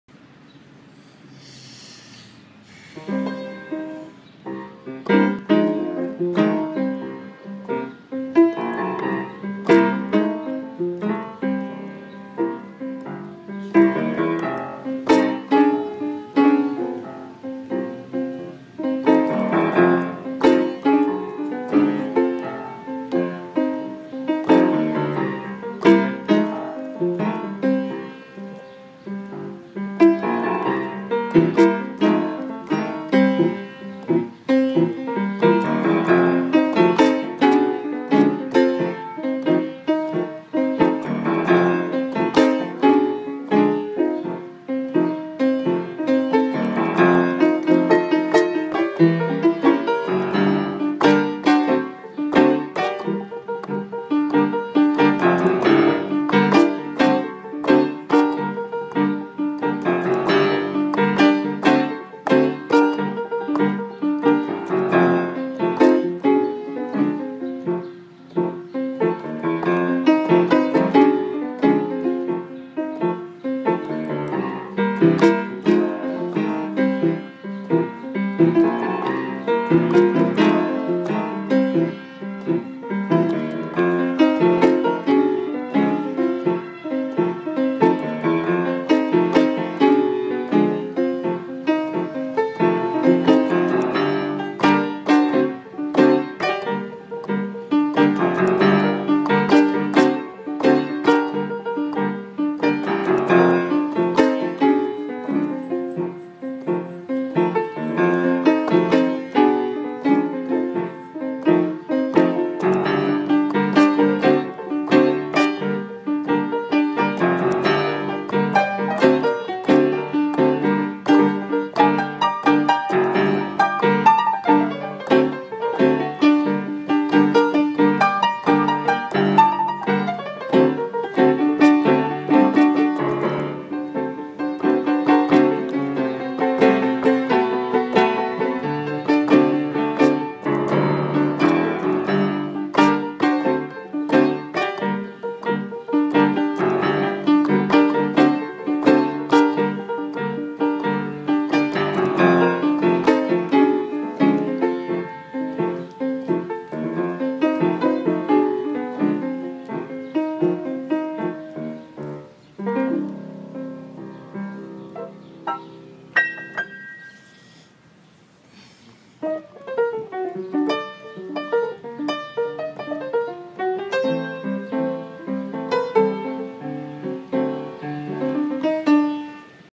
Paris Street Piano 2013 - Max - Le Jardin Anna Marly (75014)
Bluesy improvisation